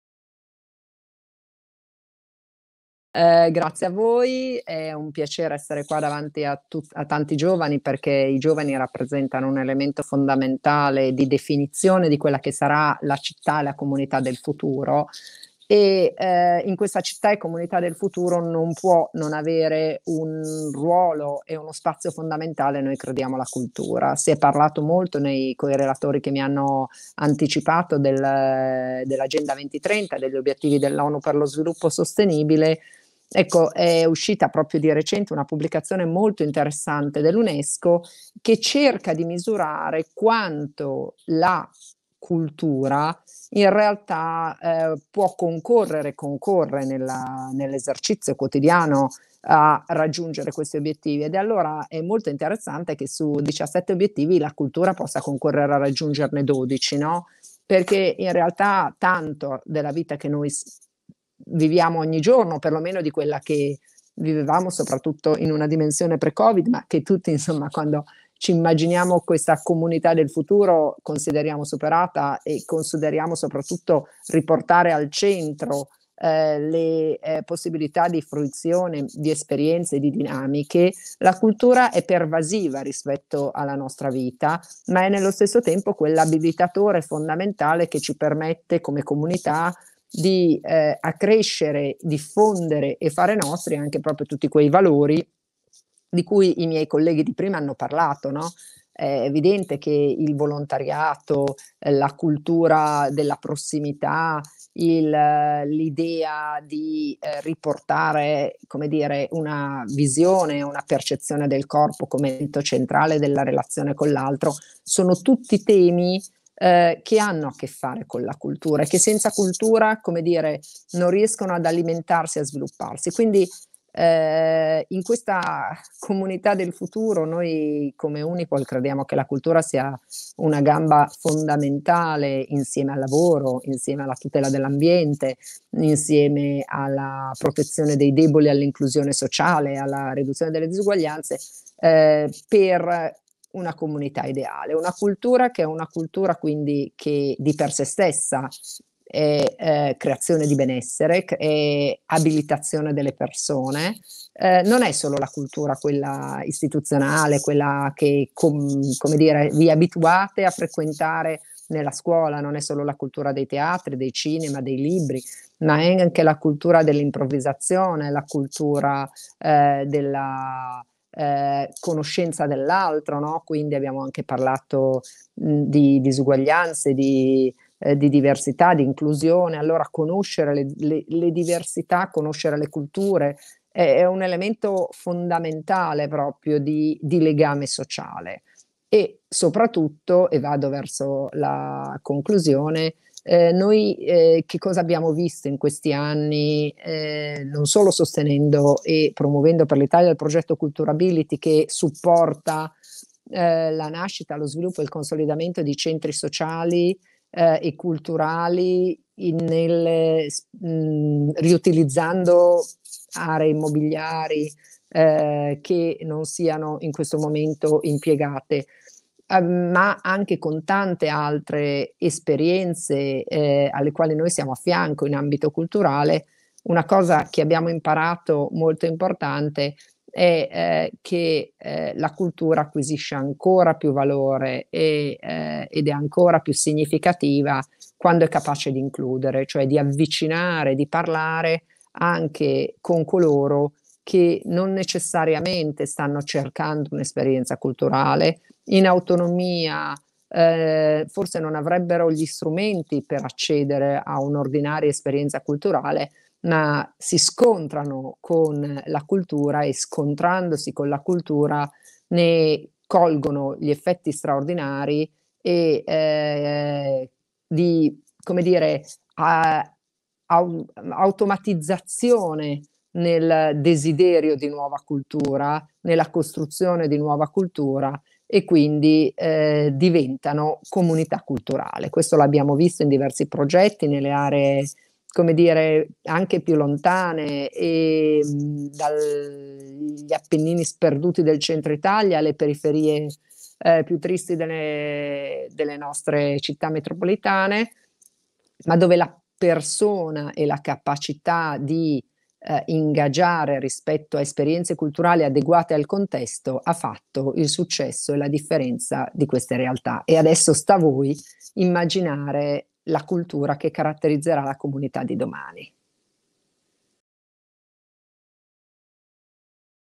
Marisa Parmigiani, Unipol Group in VocediComunità​ per la rigenerazione urbana
Può esistere una comunità senza cultura?
La risposta è semplice: no.

Perché la cultura, insieme al lavoro, alla tutela dei deboli, all'inclusione sociale, alla protezione dell'ambiente, è una gamba fondamentale delle comunità del futuro. Ed è anche l'abilitatore che ci consente di accrescere e diffonderne i valori.

Possiamo riassumere così l'intervento di Marisa Parmigiani, Head of sustainability and stakeholder management di Unipol Gruppo, al bootcamp VocediComunità per la rigenerazione urbana.

Ci ha raccontato come secondo una recente pubblicazione dell'Unesco la cultura concorra ogni giorno a raggiungere 12 dei 17 obiettivi di Sviluppo Sostenibile definiti dall'ONU.
Perché la cultura "è di per se stessa creazione di benessere".

E acquisisce ancora più valore "quando è capace di includere anche coloro che non la stanno cercando". Persone che si scontrano con la cultura, desiderano nuova cultura e diventano comunità culturali.